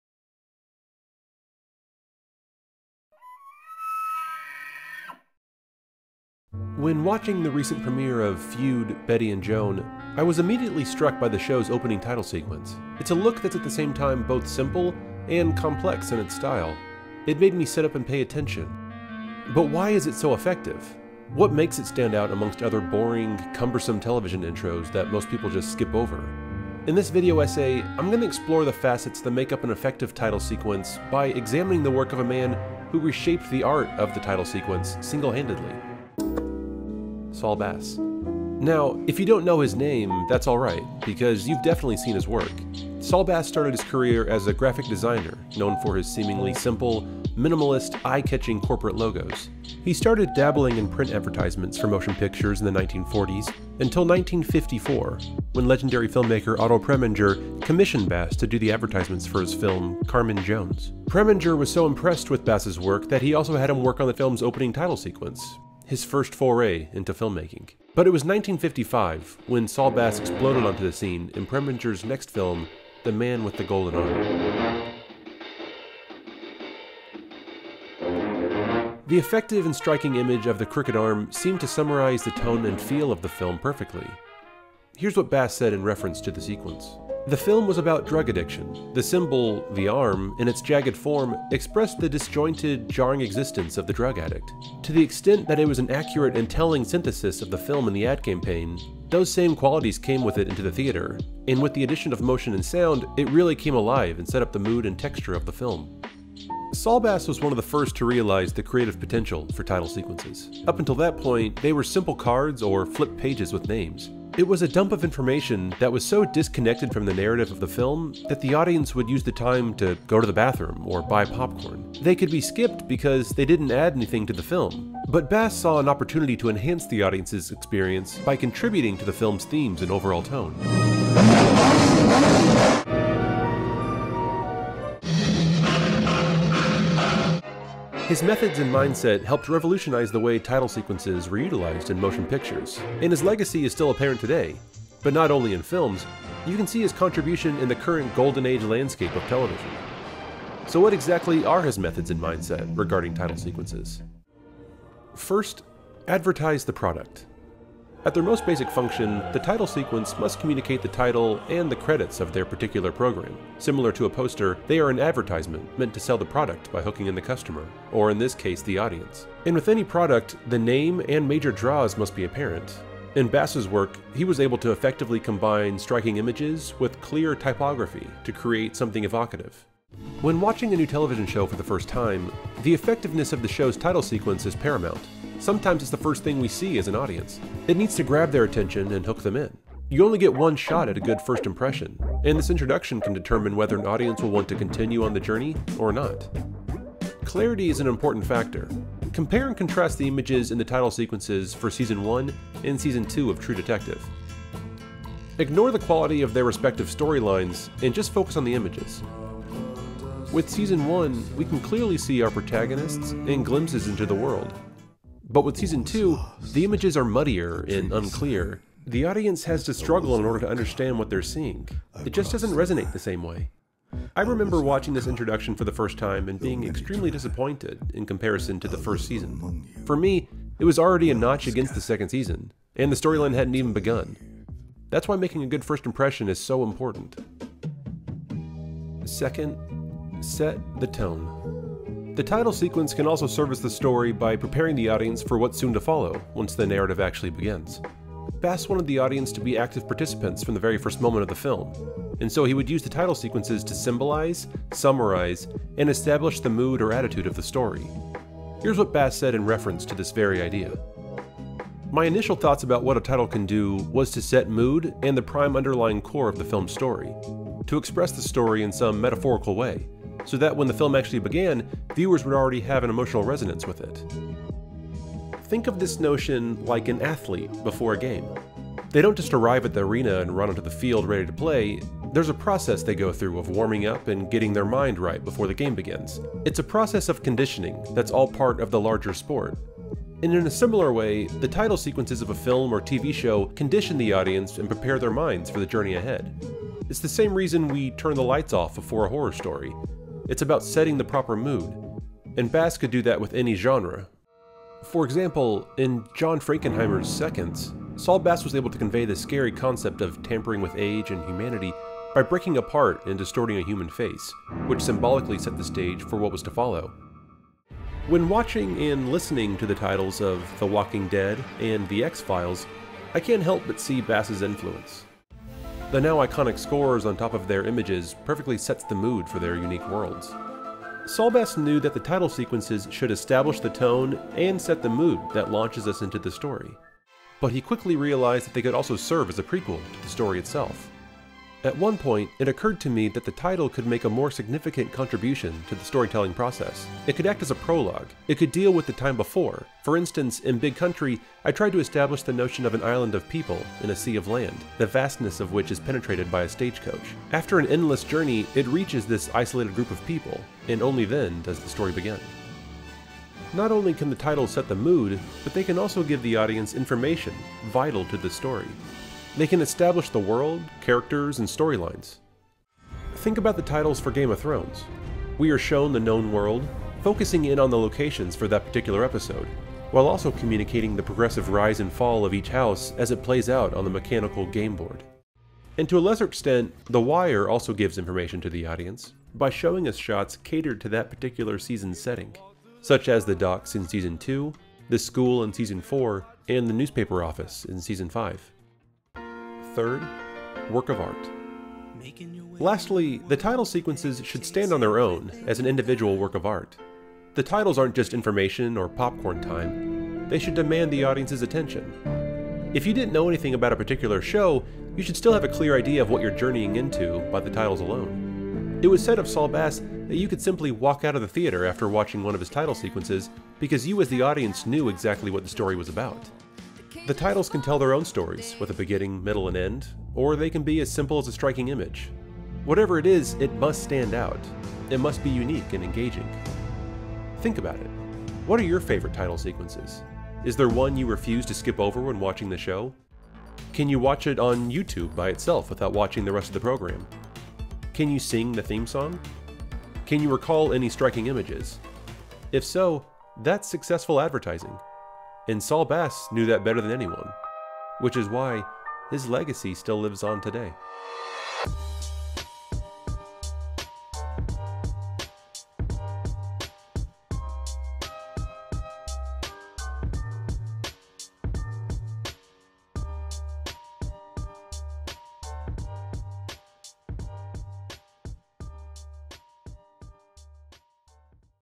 Saul Bass' Legacy | TV Title Sequences
Examining Saul Bass and his lasting legacy in title sequences by reflecting on his work in feature films and how he helped influence the current Golden Age of Television.